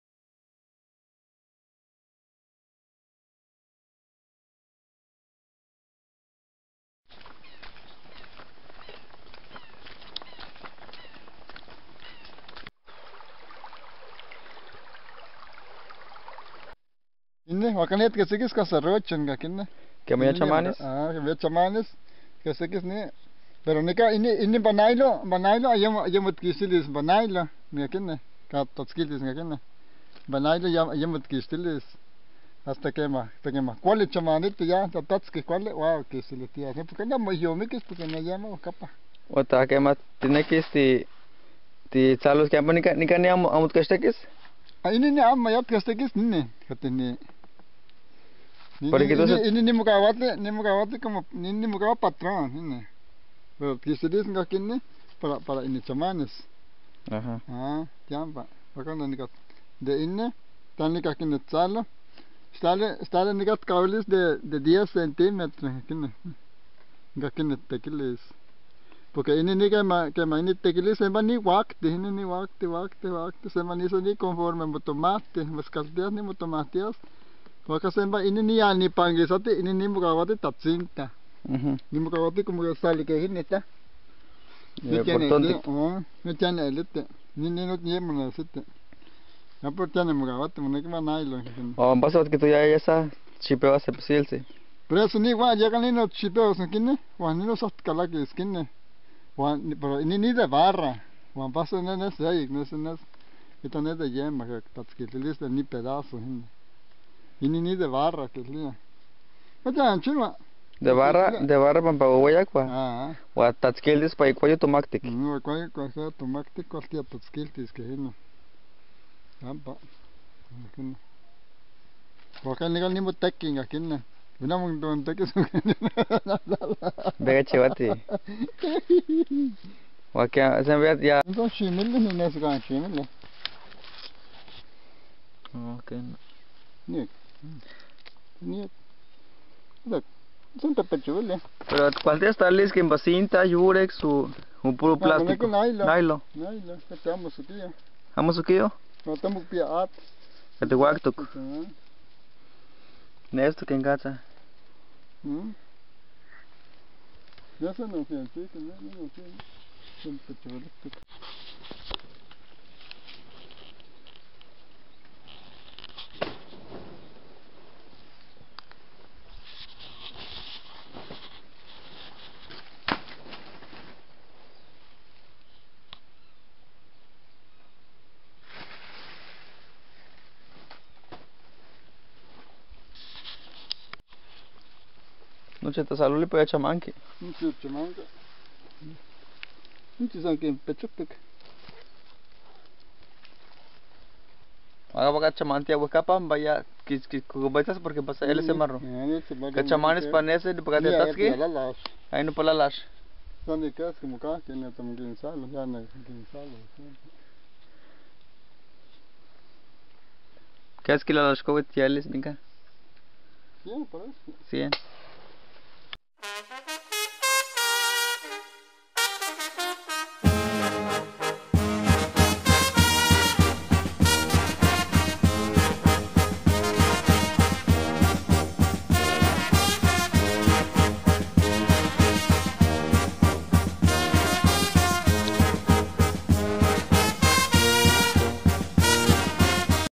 Injertos de naranja
Con una conversación Nahuatl huasteco.